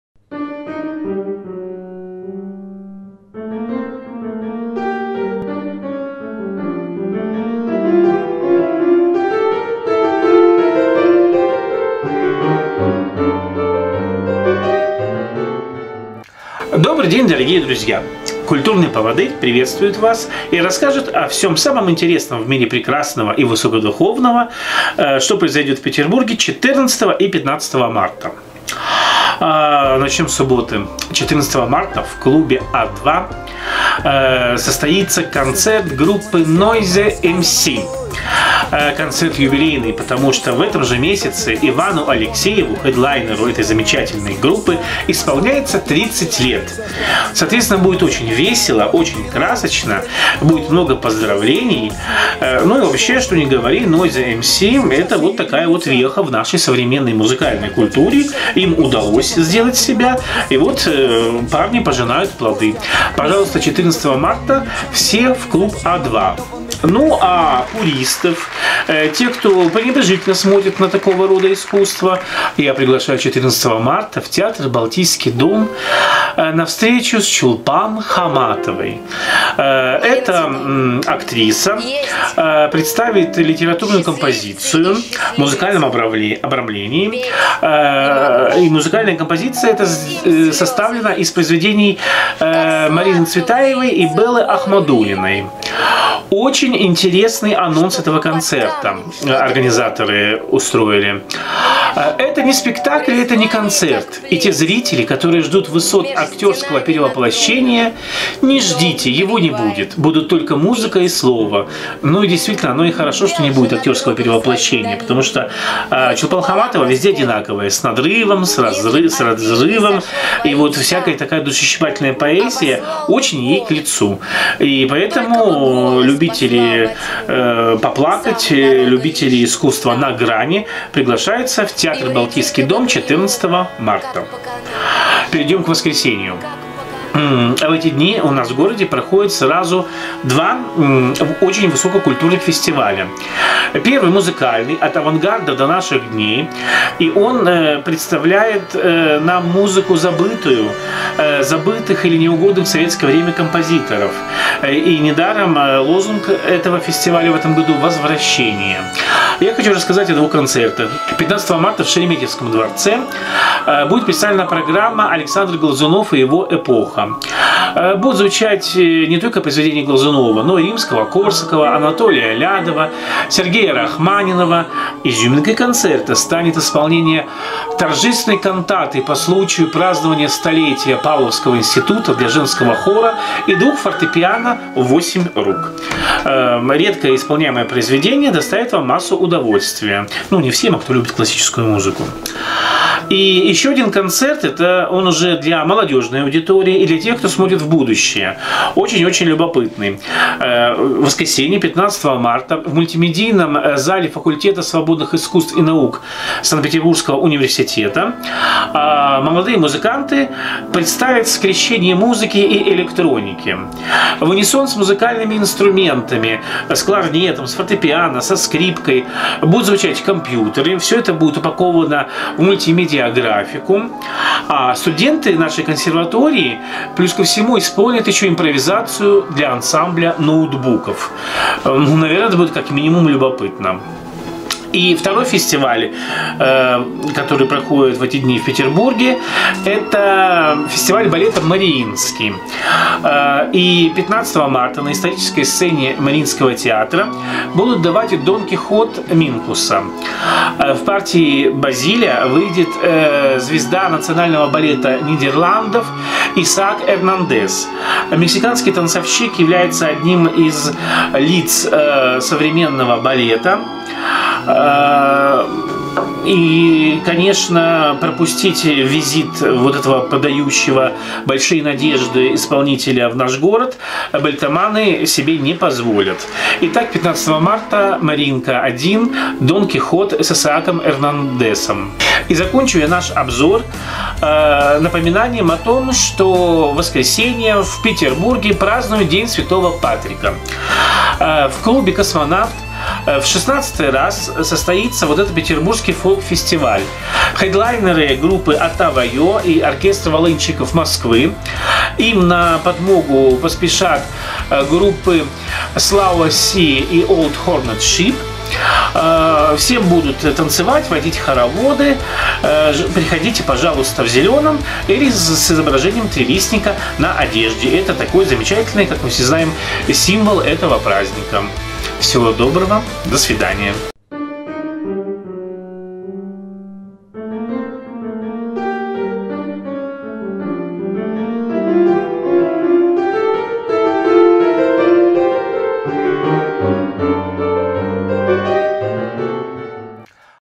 Культурный поводырь: 14-15 марта
Наступившие выходные полны самых разнообразных культурных событий. Любители тонких материй могут отправиться на встречу с актрисой Чулпан Хаматовой, поклонники балета не пропустят представления фестиваля "Мариинский", а для классических меломанов приготовил сюрпризы фестиваль "От авангарда до наших дней". Самым же веселым и энергичным зрителям - прямая дорожка в клуб "Космонавт", где будут отмечать день святого Патрика.